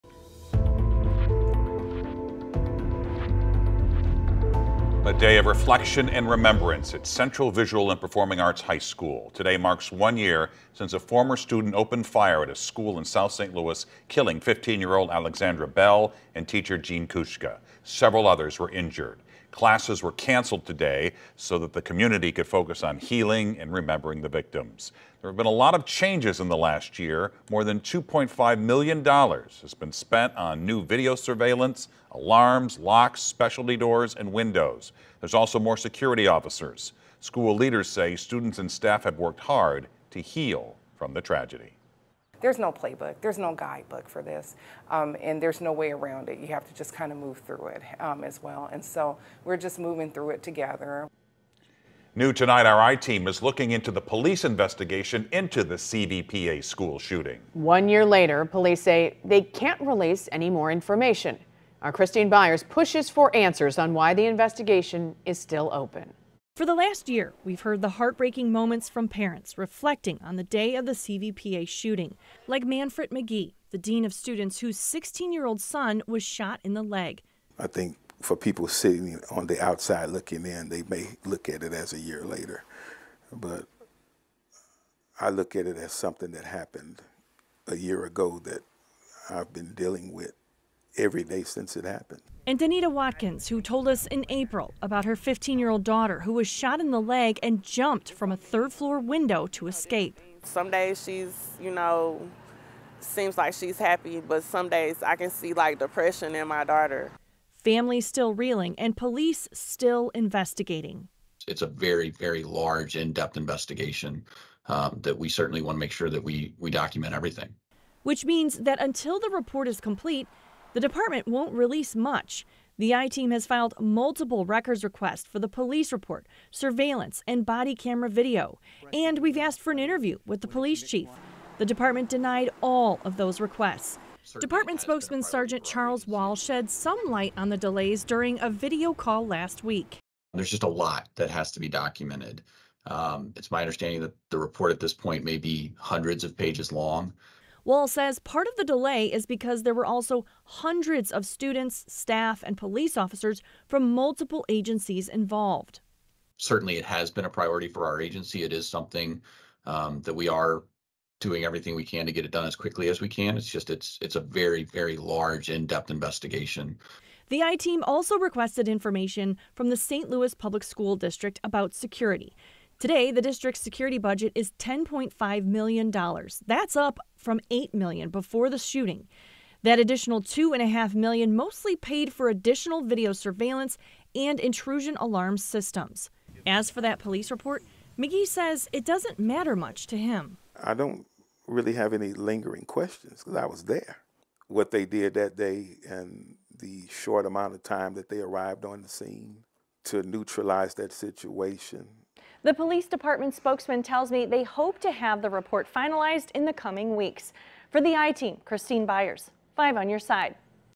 Why the police investigation into high school shooting 1 year ago is still not finished
The I-Team has filed multiple records requests for surveillance and body camera video from the day of the shooting as well as asked for an interview with St. Louis Police Chief Robert Tracy. All of those requests have been denied.

A police spokesperson said the incident report is hundreds of pages long and could be finalized in the coming weeks.